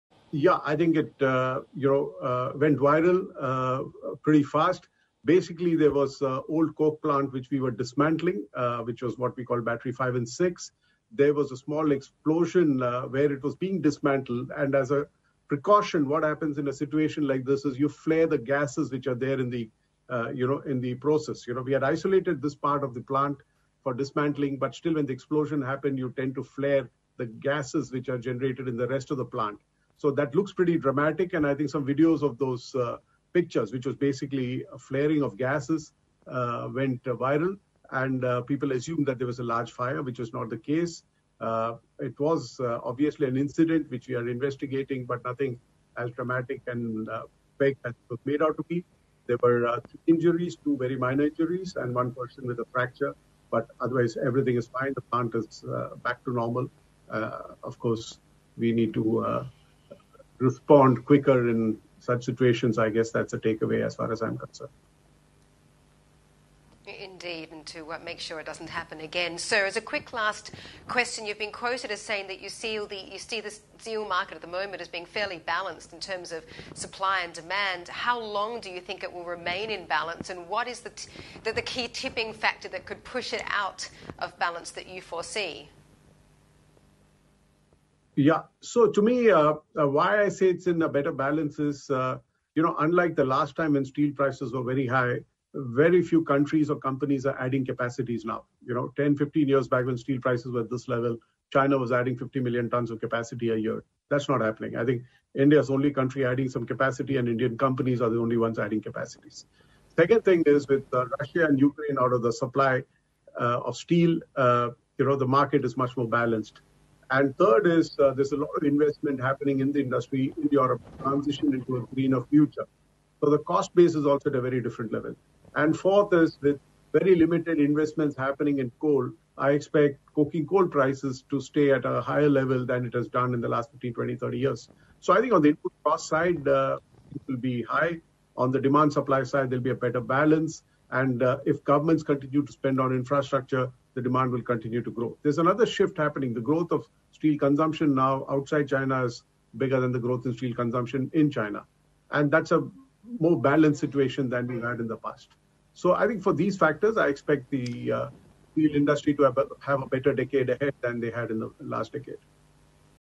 The decade ahead for the steel industry will probably be better than the last, says Tata Steel
T. V. Narendran of the steelmaker says the industry is now more balanced.